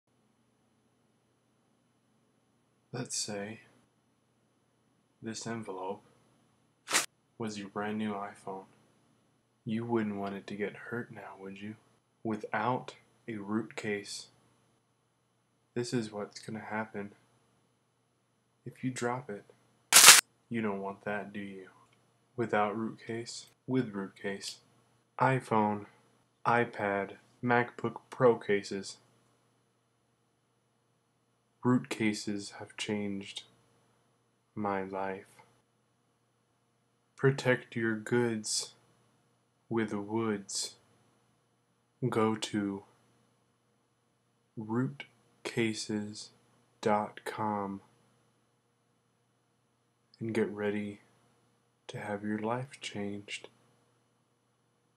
Root Cases Wood iPhone Cases
You need a Root Case (according to this edgy fan).  What do you think of his creepy comments?